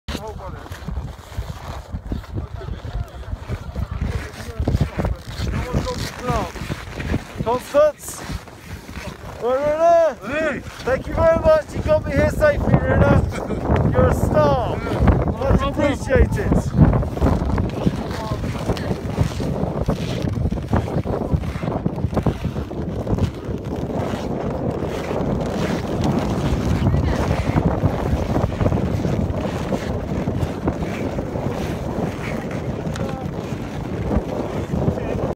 South Pole Video
Video of everyone at the south pole. Filmed by Rune Gjeldnes